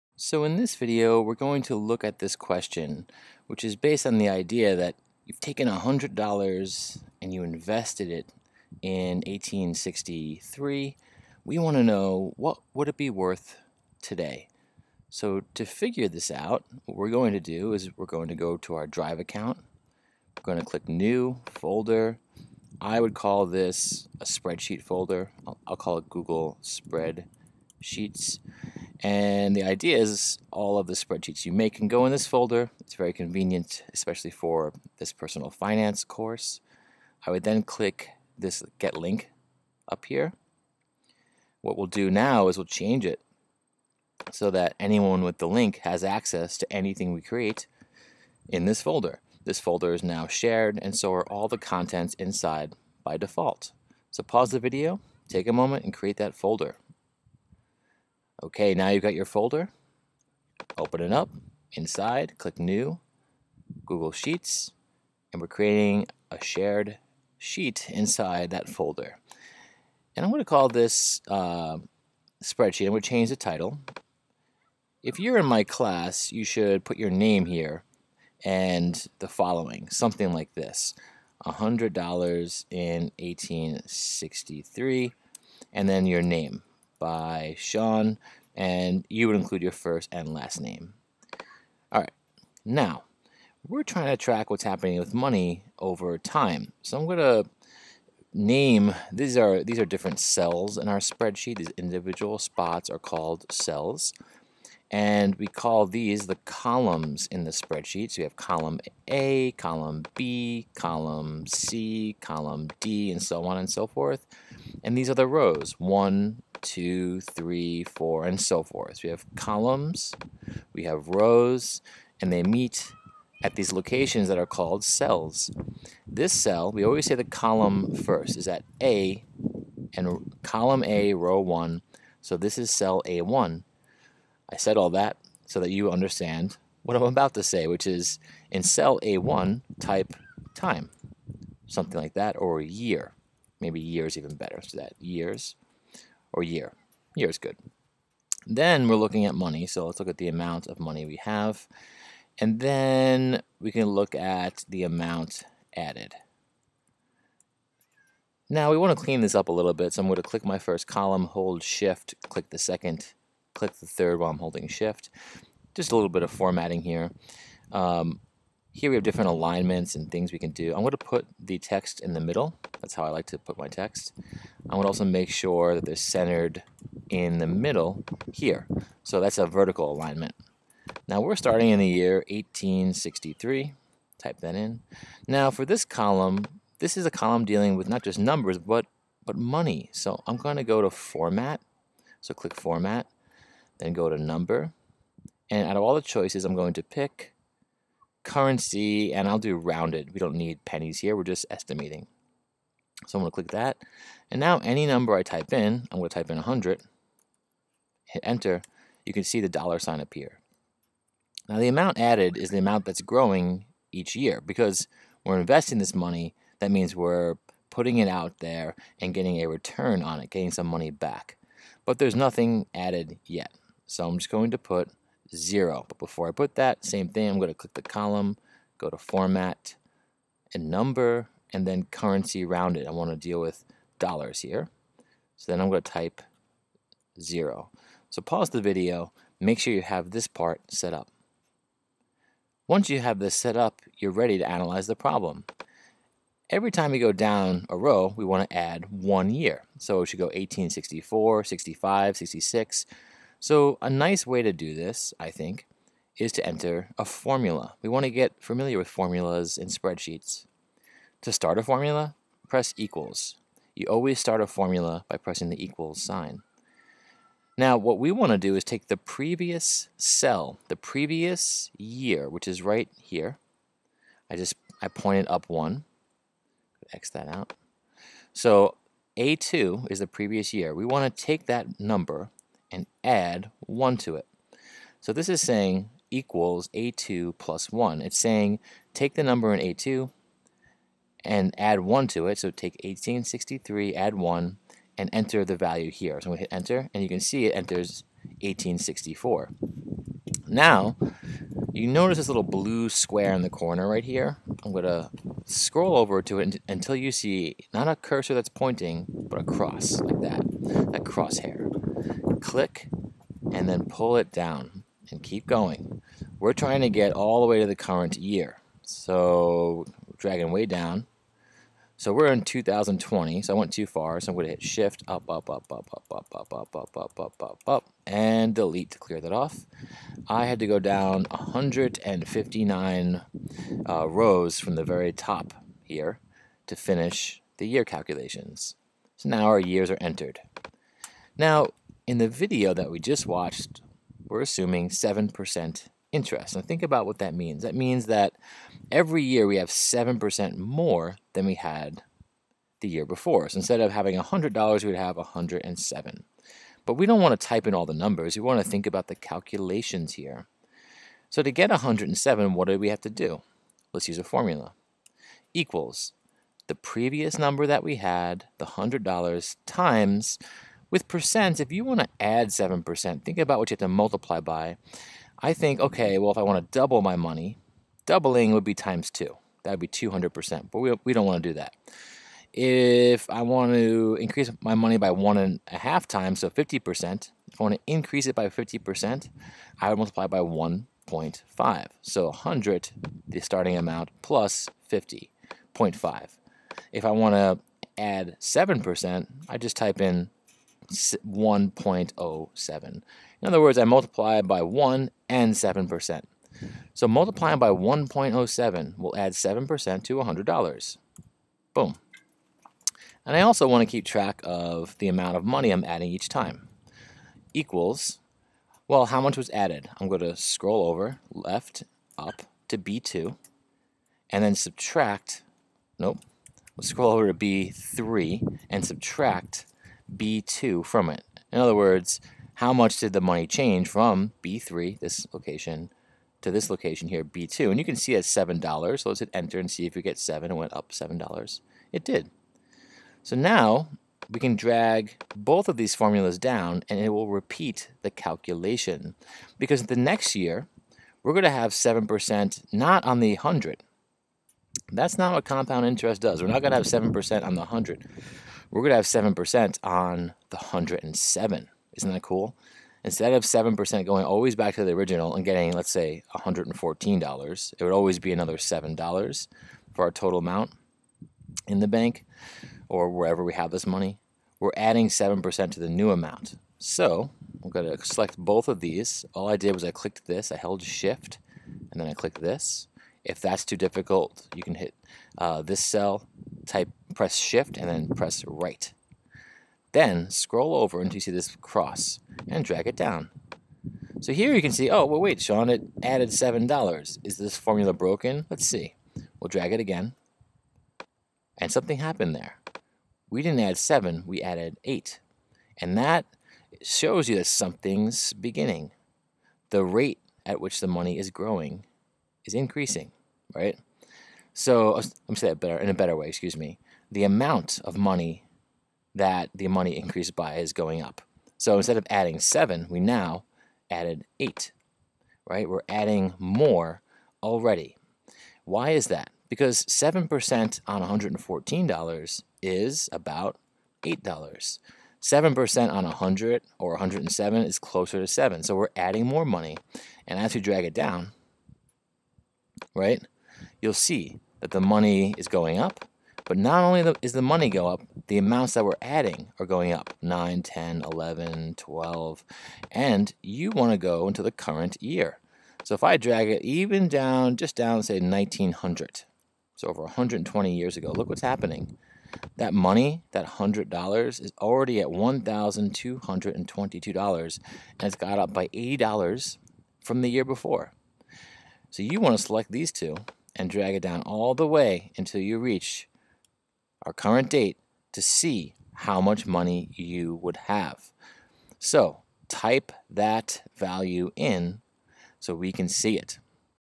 $100 in 1863
If you invested $100 in 1863 with 7% interest, what would it be worth today?

Note: this is part of an assignment for students, so the answer is left out. If you want to know the answer, just reach out and I will hare it.